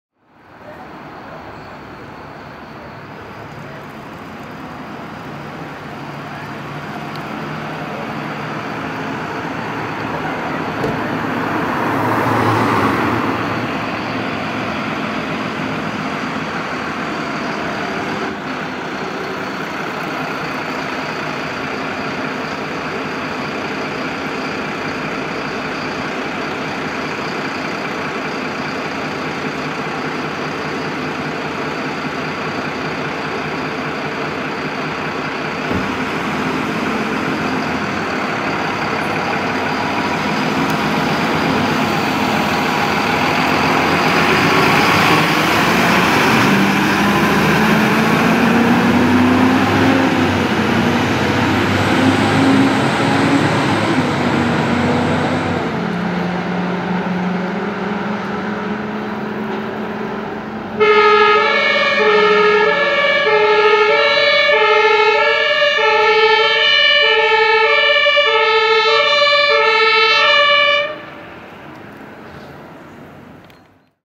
HTLF-Technik der Berufsfeuerwehr Osnabrück
Alarm für die Berufsfeuerwehr der Stadt Osnabrück. Von der Berufsfeuerwache rückte das Hilfeleistungstanklöschfahrzeug HTLF der Technik-Gruppe (1-48/2) zu einem einsatz aus. Einsatzgrund war unterstützung des Polizei.